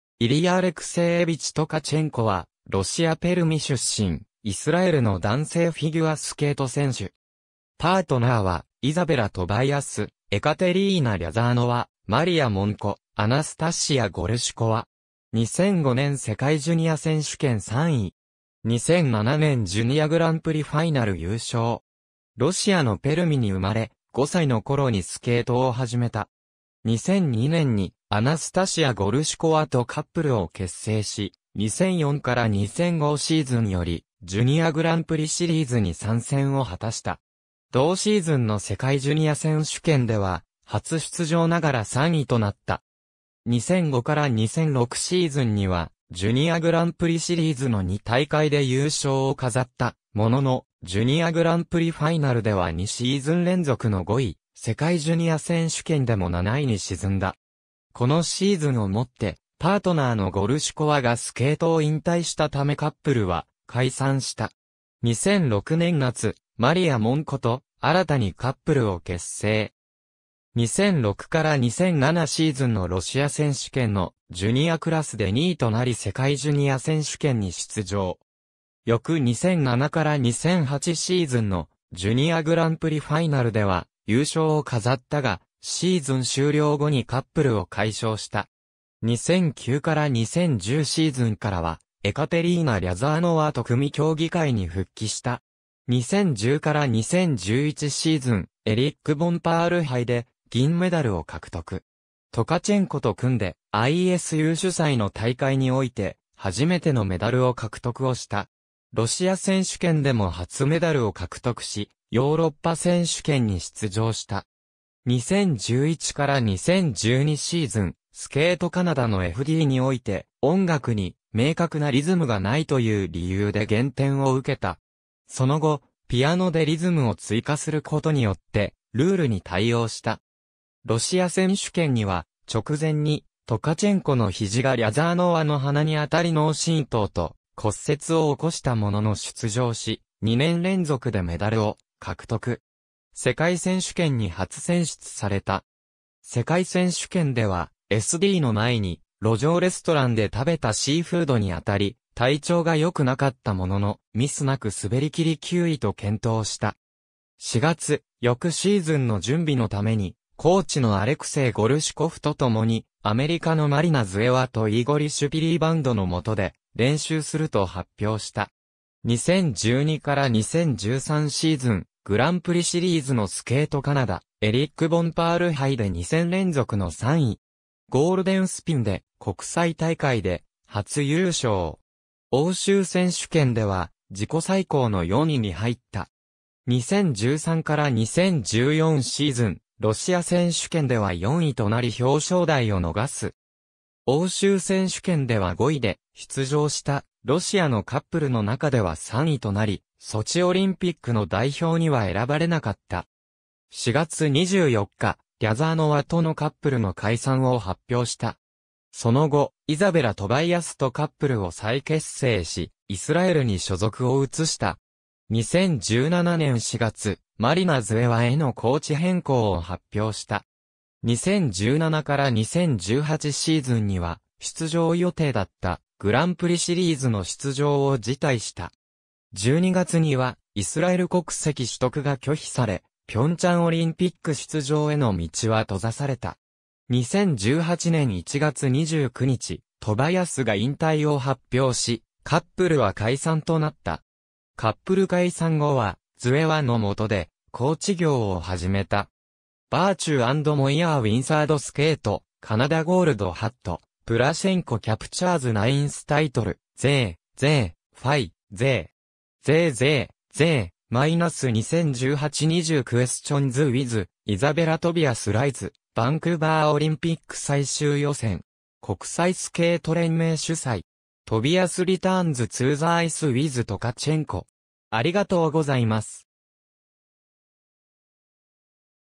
イリヤ・トカチェンコ
イリヤ・アレクセーエヴィチ・トカチェンコ（ロシア語: Илья Алексеевич Ткаченко、ロシア語ラテン翻字: Ilia Alekseevich Tkachenko、1986年12月26日 - ）は、ロシアペルミ出身、イスラエルの男性フィギュアスケート(アイスダンス)選手。
パートナーはイザベラ・トバイアス、エカテリーナ・リャザーノワ、マリア・モンコ、アナスタシア・ゴルシュコワ。
2005年世界ジュニア選手権3位。
2007年ジュニアグランプリファイナル優勝。
ロシアのペルミに生まれ、5歳のころにスケートを始めた。
2002年にアナスタシア・ゴルシュコワとカップルを結成し、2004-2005シーズンよりジュニアグランプリシリーズに参戦を果たした。
同シーズンの世界ジュニア選手権では初出場ながら3位となった。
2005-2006シーズンにはジュニアグランプリシリーズの2大会で優勝を飾ったものの、ジュニアグランプリファイナルでは2シーズン連続の5位、世界ジュニア選手権でも7位に沈んだ。
このシーズンをもってパートナーのゴルシュコワがスケートを引退したためカップルは解散した。
2006年夏、マリア・モンコと新たにカップルを結成。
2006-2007シーズンのロシア選手権のジュニアクラスで2位となり世界ジュニア選手権に出場。
翌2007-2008シーズンのジュニアグランプリファイナルでは優勝を飾ったが、シーズン終了後にカップルを解消した。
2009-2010シーズンからはエカテリーナ・リャザーノワと組み競技会に復帰した。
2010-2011シーズン、エリック・ボンパール杯で銀メダルを獲得。
トカチェンコと組んで、ISU主催の大会において初めてのメダルを獲得をした。
ロシア選手権でも初メダルを獲得し、ヨーロッパ選手権に出場した。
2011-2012シーズン、スケートカナダのFDにおいて、「音楽に明確なリズムが無い」という理由で減点を受けた。
その後、ピアノでリズムを追加することによってルールに対応した。
ロシア選手権には直前に、トカチェンコの肘がリャザーノワの鼻に当たり脳震盪と骨折を起こしたものの出場し、2年連続でメダルを獲得。
世界選手権に初選出された。
世界選手権ではSDの前に路上レストランで食べたシーフードに当たり体調が良くなかったものの、ミス無く滑りきり9位と健闘した。
4月、翌シーズンの準備のためにコーチのアレクセイ・ゴルシュコフと共に、アメリカのマリナ・ズエワとイーゴリ・シュピリバンドの元で練習すると発表した。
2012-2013シーズン、グランプリシリーズのスケートカナダ、エリック・ボンパール杯で2戦連続の3位。
ゴールデンスピンで国際大会で初優勝。
欧州選手権では自己最高の4位に入った。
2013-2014シーズン、ロシア選手権では4位となり表彰台を逃す。
欧州選手権では5位で、出場したロシアのカップルの中では3位となり、ソチオリンピックの代表には選ばれなかった。
4月24日、リャザーノワとのカップルの解散を発表した。
その後、イザベラ・トバイアスとカップルを再結成し、イスラエルに所属を移した。
2017年4月、マリナ・ズエワへのコーチ変更を発表した。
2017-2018シーズンには、出場予定だったグランプリシリーズの出場を辞退した。
12月にはイスラエル国籍取得が拒否され、平昌オリンピック出場への道は閉ざされた。
2018年1月29日、トバイアスが引退を発表し、カップルは解散となった。
カップル解散後はズエワの元でコーチ業を始めた。
^ Virtue and Moir win third Skate Canada gold ^ Plushenko Captures Ninth Title ^ Екатерина Рязанова: "Расстраиваться нет смысла:все, что мы могли, мы сделали" ^ Работа в группе Шпильбанда/Зуевой пойдет на пользу - фигурист Ткаченко ^ Фигуристы Екатерина Рязанова
и Илья Ткаченко больше не будут выступать вместе ^ Тобиас и Ткаченко собираются выступать за ...